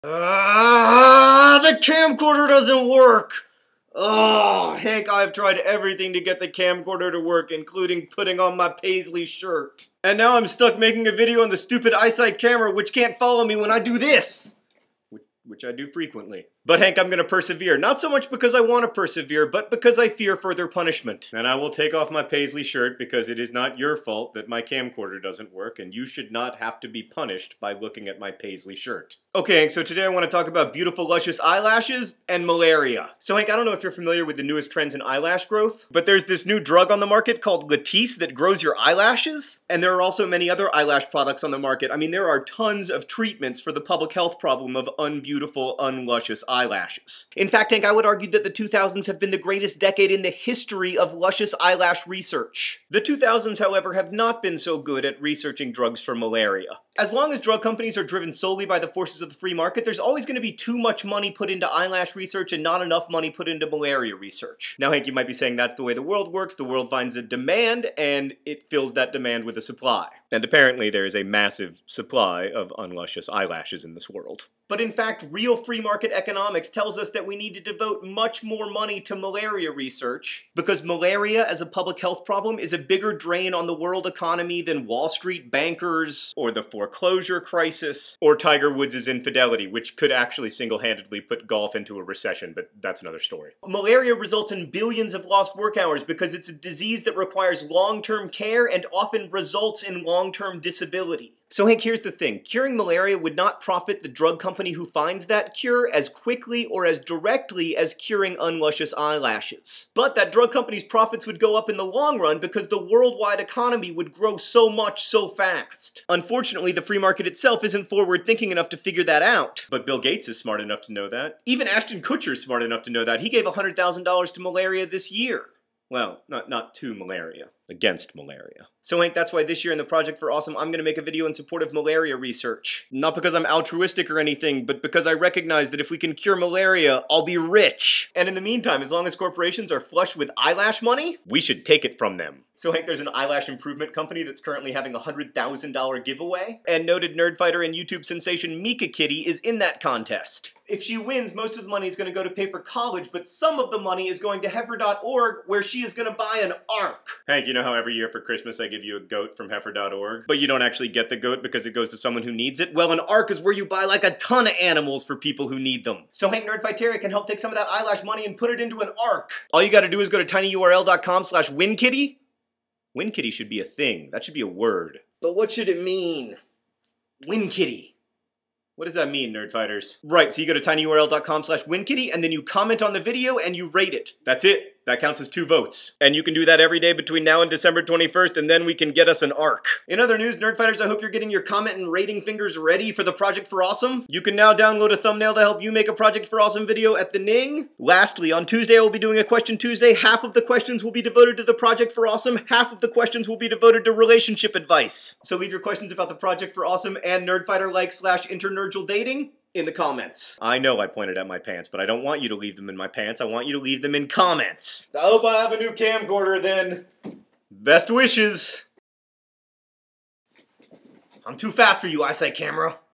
Eyelash Malaria
In which John discusses medications that thicken and lengthen eyelashes, malaria, and priorities of the free market in determining what sorts of drugs the pharmaceutical industry should develop.